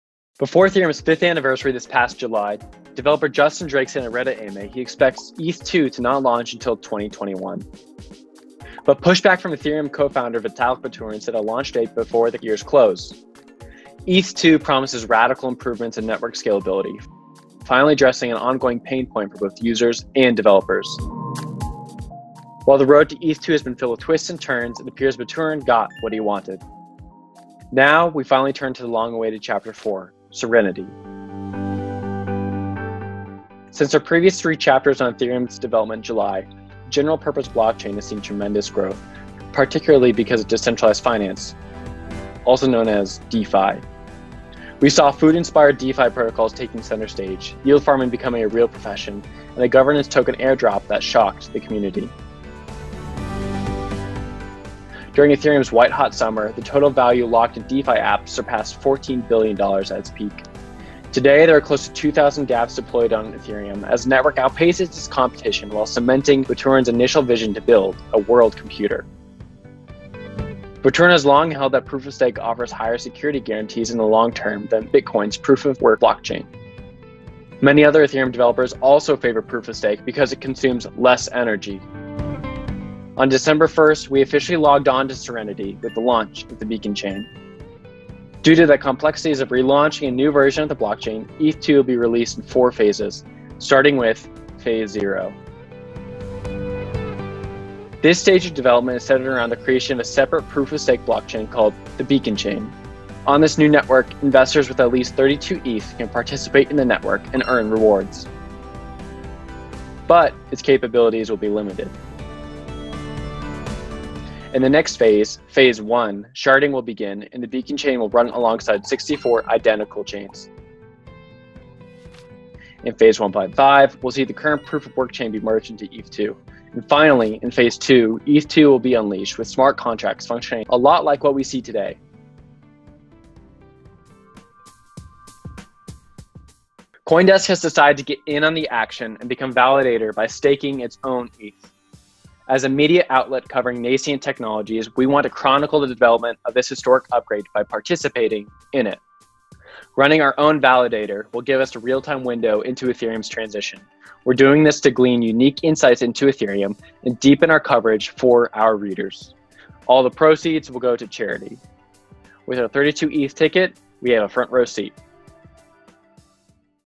Ethereum 2.0 Explained | CoinDesk Stakes 32 ETH on Beacon Chain
On Dec. 1, we officially logged in to the beginnings of Ethereum 2.0 with the networks Serenity upgrade, marking the beginnings of a transition to a proof-of-stake consensus mechanism. Just like the launch of Ethereum, this upgrade will be deployed in four phases.

CoinDesk tech reporter Will Foxley explains why the developers favored proof-of-stake, the phases of deployment and why CoinDesk is staking in Ethereum 2.0.

CoinDesk Podcast Network: spotify:show:2jyIhkvAQhed8AWDZiPpSP
Markets Daily Crypto Roundup: spotify:show:7sDXM8BlxsUqzL2IqmLqwE